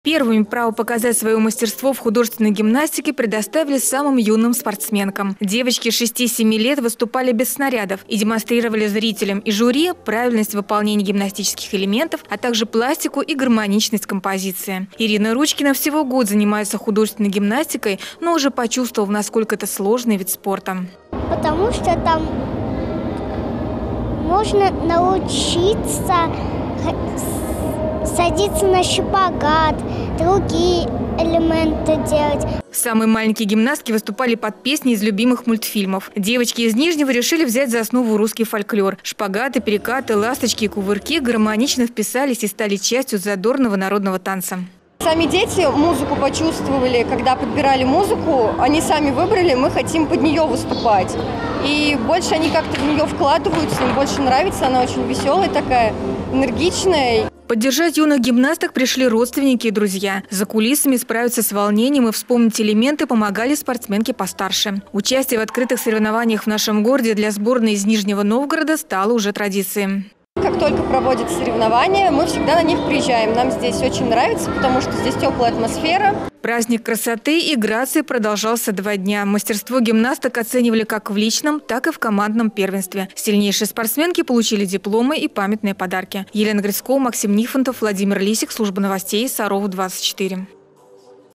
Осенний карнавал - 2018
В нашем городе прошло открытое первенство по художественной гимнастике «Осенний карнавал». Свое мастерство владения телом и спортивными снарядами показали более 80 гимнасток из Сарова, Арзамаса и Нижнего Новгорода.